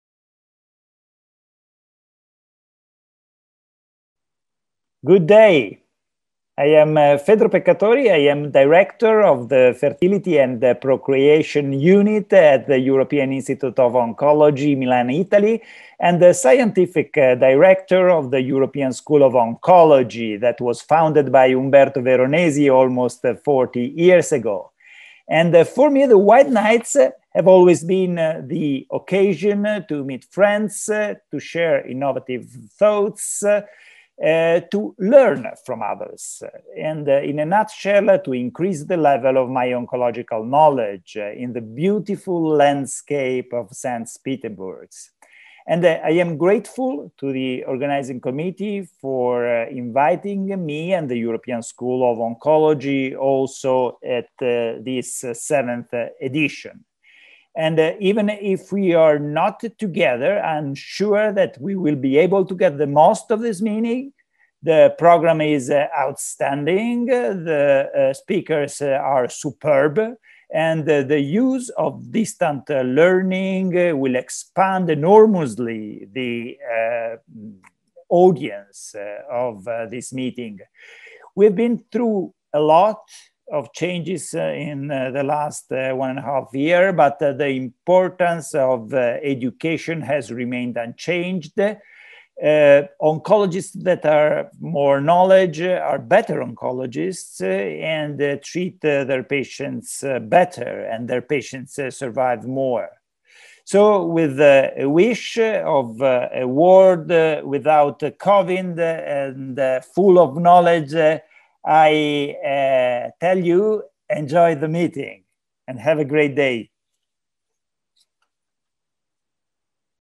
Обращение к участникам форума Fedro Peccatori, директора по научным исследованиям ESO
Добрый день! Меня зовут Федро Пеккатори. Я – директор отделения фертильности и репродукции Европейского института онкологии в Милане, Италия, а также директор по научным исследованиям Европейской школы онкологии, основанной почти 40 лет назад Умберто Веронези. Для меня форум «Белые ночи» всегда был возможностью встретить друзей, обменяться инновационными мыслями, обменяться опытом, повысить уровень знаний в области онкологии, при этом находясь в таком красивом городе как Санкт-Петербург. Я благодарен организационному комитету за то, что меня и Европейскую школу онкологии пригласили на этот седьмой по счету форум. И хотя мы не вместе, я уверен, что мы сможем получить максимальную пользу от данного мероприятия. Программа – превосходная, докладчики – отличные, а дистанционный формат позволит значительно увеличить количество участников. За последние полтора года мы пережили немало перемен, но важность образования осталась неизменной. Врачи-онкологи, которые владеют большим количеством знаний, являются более сильными специалистами и лучше лечат пациентов, которые имеют больше шансов выжить. Поэтому, с надеждой на мир, в котором нет ковида, но царят знания, желаю вам получить удовольствие от форума и отличного дня!